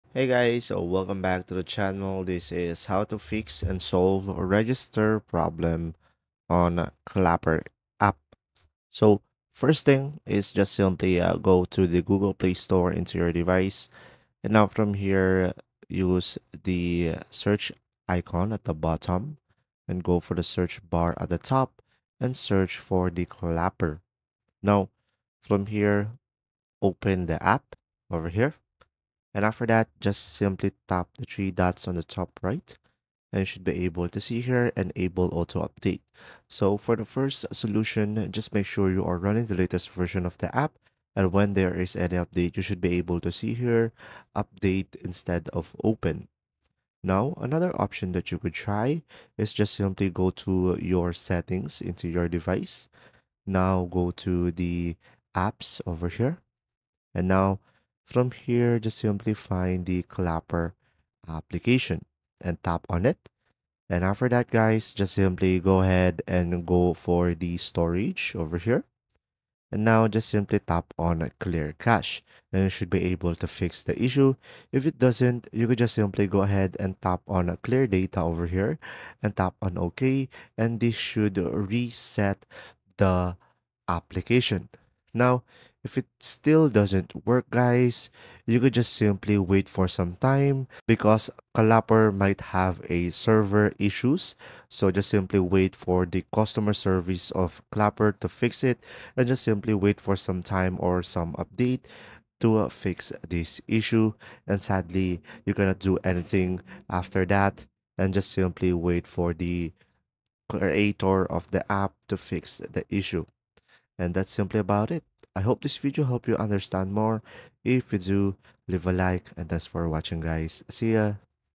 How to fix and solve register problem on Clapper app - Problem Solved (Works 2026)
How to fix and solve register problem on Clapper app - Problem Solved!

Are you having trouble registering on the Clapper App? Don't worry, you're not alone! Many users have reported issues during the sign-up process, but in this video, we've got the solution for you! We'll walk you through the steps to overcome the Clapper App register problem and get you started with the app in no time. Whether you're new to Clapper or just need a quick fix, this video is for you!